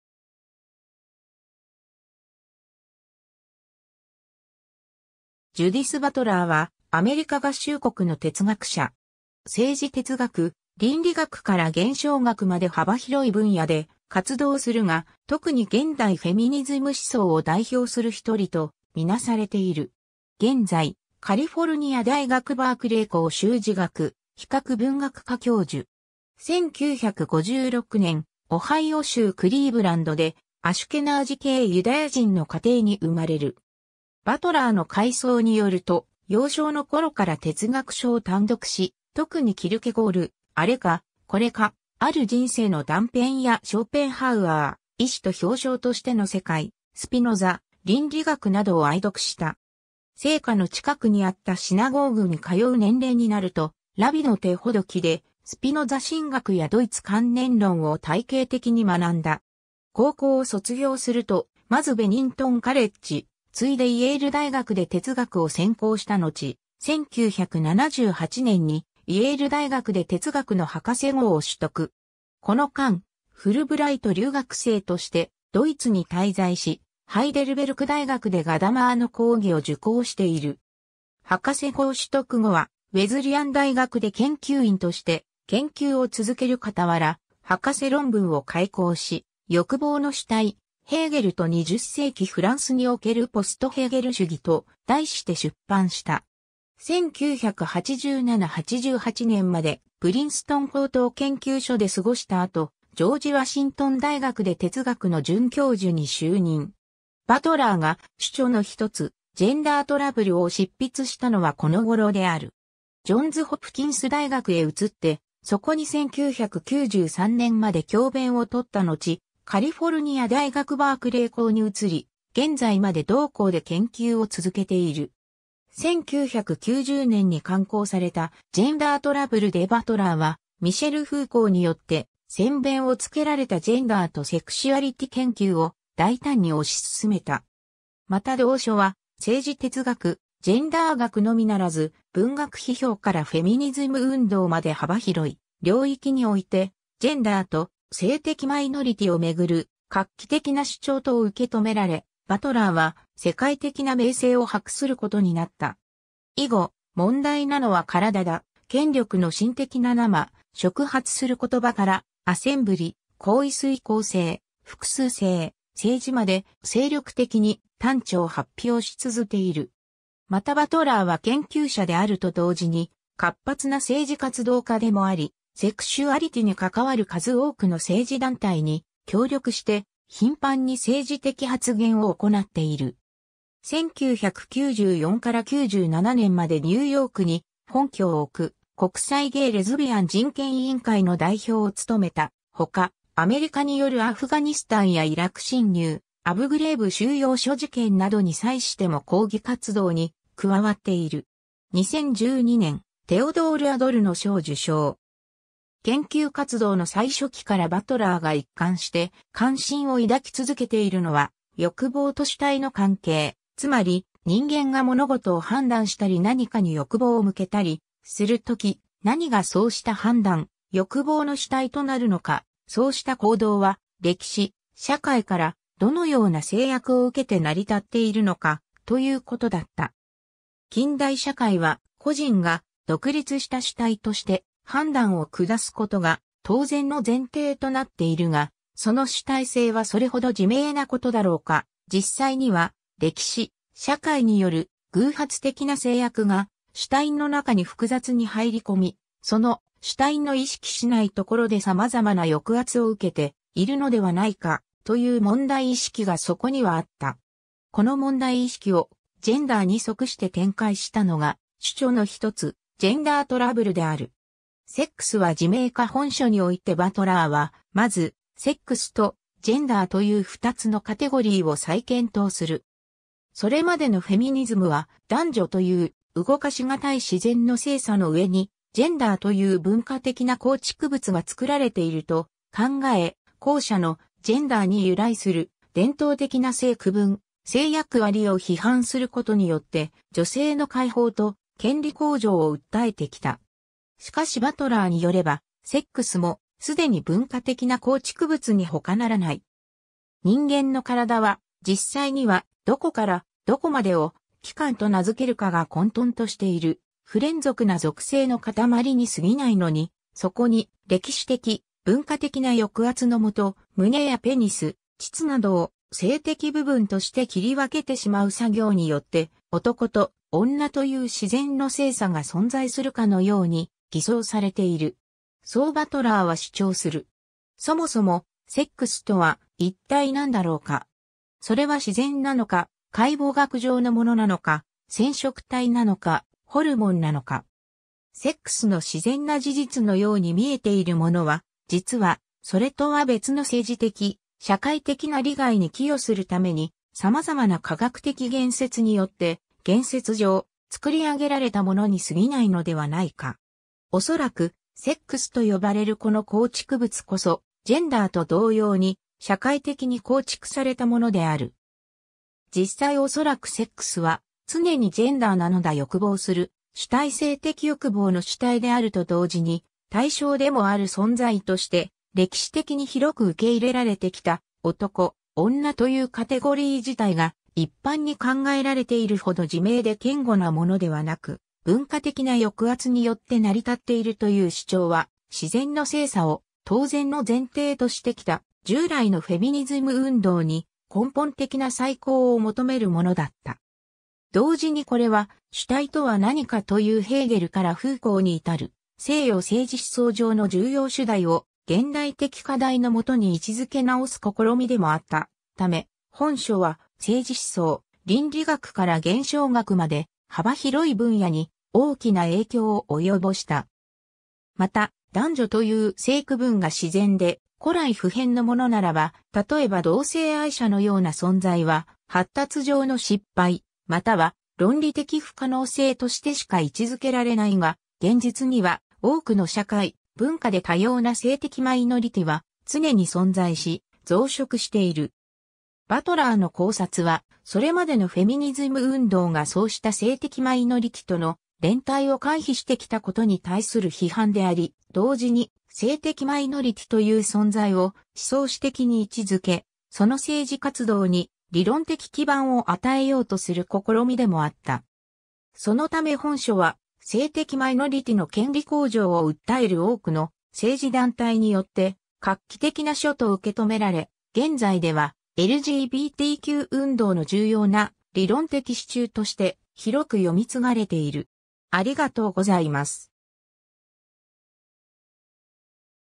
ジュディス・バトラー
ジュディス・バトラー（Judith P. Butler、1956年2月24日 - ）は、アメリカ合衆国の哲学者。
 政治哲学・倫理学から現象学まで幅広い分野で活動するが、とくに現代フェミニズム思想を代表する一人とみなされている。
現在、カリフォルニア大学バークレー校修辞学・比較文学科教授。
 1956年、オハイオ州クリーヴランドでアシュケナージ系ユダヤ人（迫害を逃れドイツ語圏・旧東欧諸国に移住したユダヤ人）の家庭に生まれる。
バトラーの回想によると幼少の頃から哲学書を耽読し、とくにキルケゴール『あれか、これか：ある人生の断片』やショーペンハウアー『意志と表象としての世界』、スピノザ『倫理学』などを愛読した。
 生家の近くにあったシナゴーグに通う年齢になると、ラビの手ほどきでスピノザ神学やドイツ観念論を体系的に学んだ。
高校を卒業すると、まずベニントン・カレッジ、ついでイェール大学で哲学を専攻したのち、1978年にイェール大学で哲学の博士号を取得。
この間、フルブライト留学生としてドイツに滞在し、ハイデルベルク大学でガダマーの講義を受講している。
 博士号取得後はウェズリアン大学で研究員として研究をつづけるかたわら、博士論文を改稿し『欲望の主体：ヘーゲルと二〇世紀フランスにおけるポスト・ヘーゲル主義』と題して出版した。
1987−88年までプリンストン高等研究所で過ごしたあと、ジョージ・ワシントン大学で哲学の准教授に就任。
バトラーが主著のひとつ『ジェンダー・トラブル』を執筆したのはこの頃である。
ジョンズ・ホプキンス大学へ移ってそこに1993年まで教鞭をとったのちカリフォルニア大学バークレー校に移り、現在まで同校で研究を続けている。
 1990年に刊行された『ジェンダー・トラブル』でバトラーは、ミシェル・フーコーによって先鞭をつけられたジェンダーとセクシュアリティ研究を大胆に推し進めた。
また同書は政治哲学・ジェンダー学のみならず、文学批評からフェミニズム運動まで幅広い領域においてジェンダーと性的マイノリティをめぐる画期的な主張と受け止められ、バトラーは世界的な名声を博することになった。
 以後『問題なのは身体だ』（未邦訳 1993）、『権力の心的な生』『触発する言葉』（ともに1997）から『アセンブリ―行為遂行性・複数性・政治』(2015)まで精力的に単著を発表しつづている。
 またバトラーは研究者であると同時に活発な政治活動家でもあり、セクシュアリティに関わる数多くの政治団体に協力して頻繁に政治的発言を行っている。
1994〜97年までニューヨークに本拠を置く「国際ゲイ・レズビアン人権委員会」の代表を務めたほか、アメリカによるアフガニスタンやイラク侵入、アブグレイブ収容所事件などに際しても抗議活動に加わっている。
2012年テオドール・アドルノ賞受賞。
 研究活動の最初期からバトラーが一貫して関心を抱き続けているのは、欲望と主体の関係、つまり人間が物事を判断したり何かに欲望を向けたりするとき、何がそうした判断・欲望の主体となるのか、そうした行動は歴史・社会からどのような制約を受けて成り立っているのか、ということだった。
近代社会は個人が「独立した主体として判断をくだす」ことが当然の前提となっているが、その主体性はそれほど自明なことだろうか、実際には歴史・社会による偶発的な制約が「主体」の中に複雑に入り込み、その「主体」の意識しないところでさまざまな抑圧を受けているのではないか、という問題意識がそこにはあった。
 この問題意識を「ジェンダー」に即して展開したのが主著のひとつ『ジェンダー・トラブル』である。
 セックスは自明か 本書においてバトラーは、まず「セックス」と「ジェンダー」という二つのカテゴリーを再検討する。
それまでのフェミニズムは、「男女」という動かしがたい自然の性差（セックス）の上に「ジェンダー」という文化的な構築物が作られていると考え、後者の「ジェンダー」に由来する伝統的な性区分・性役割（「男は外で働き・女は家庭を守る」「男は強く・女は優しい」など）を批判することによって、女性の解放と権利向上を訴えてきた。
 しかしバトラーによれば、...